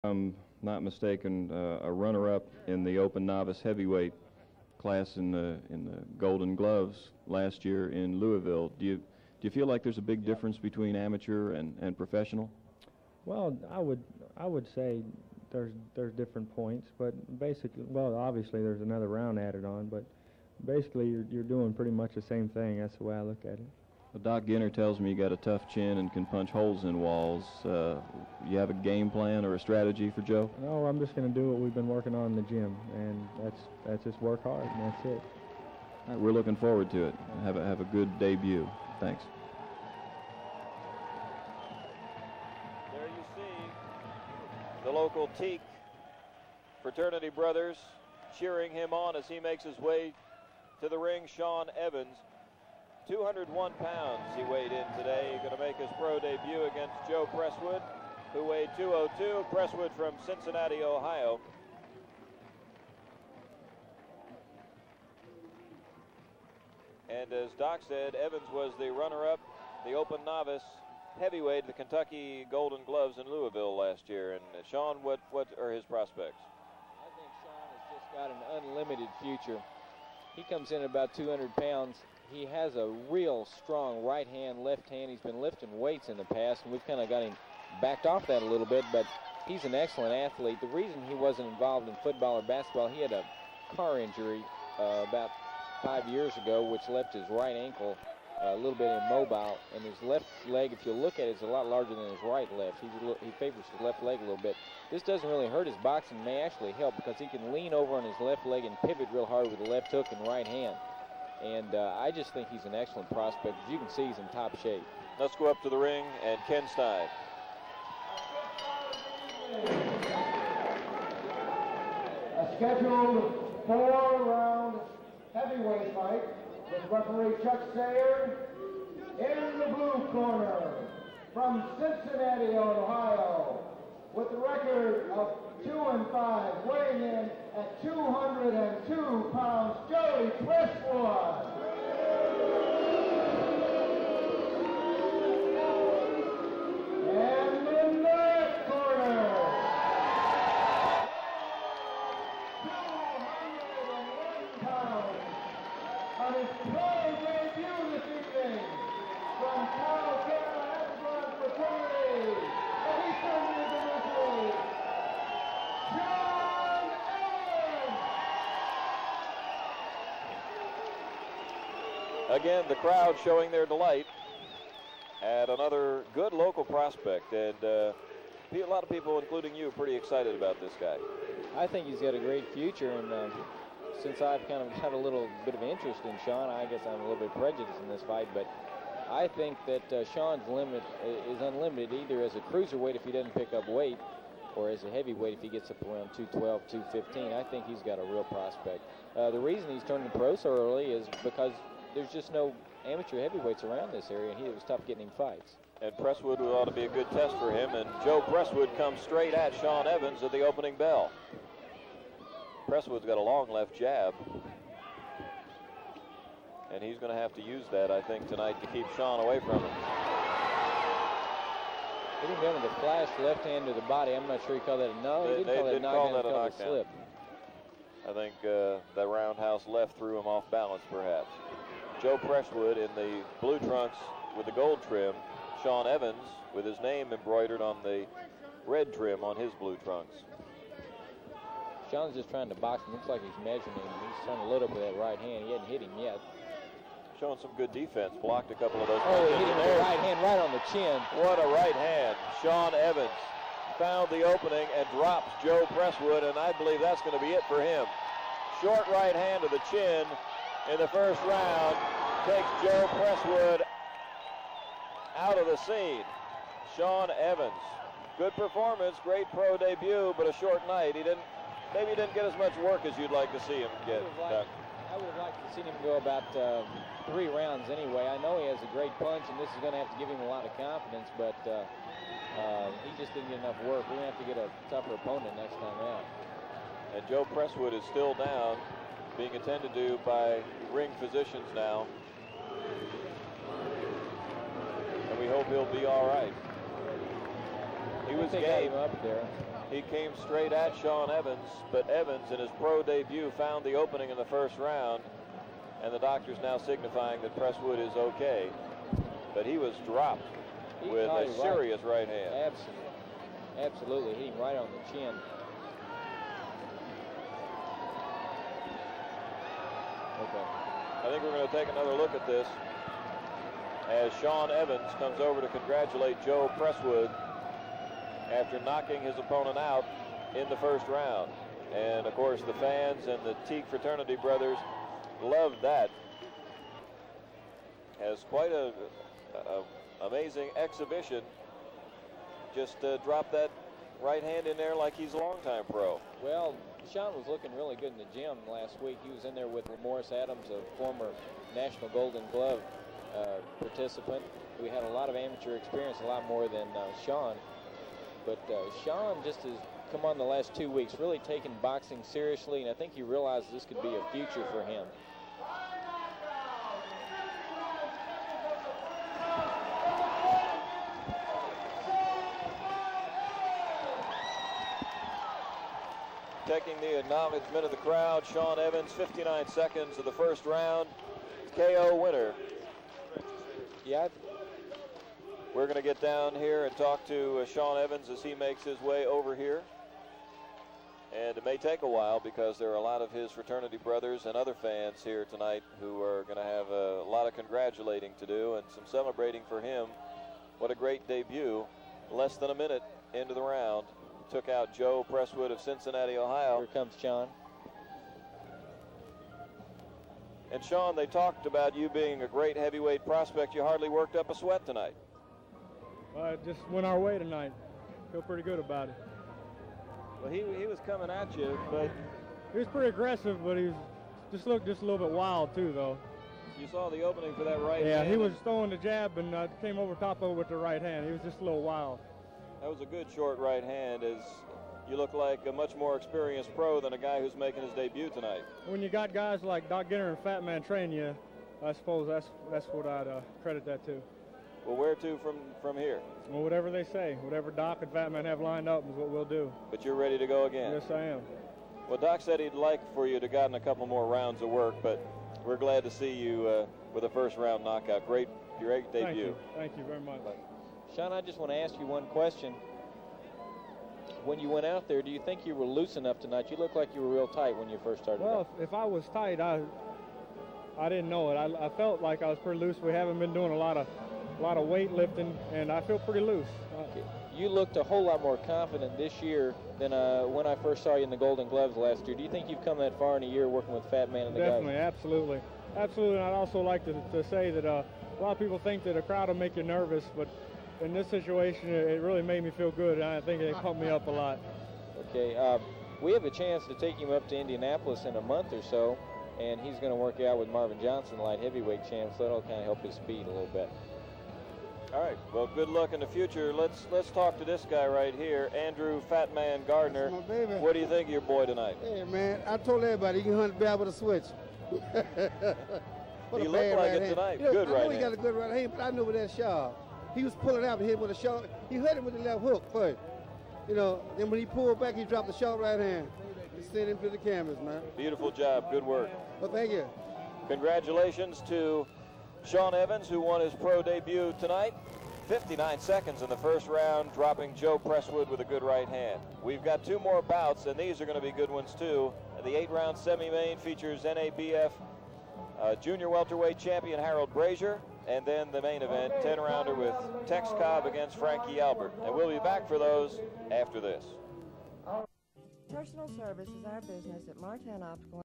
Shawn Evans 59 second face first KO with spit bubbles in professional debut.
59 second knockout in professional debut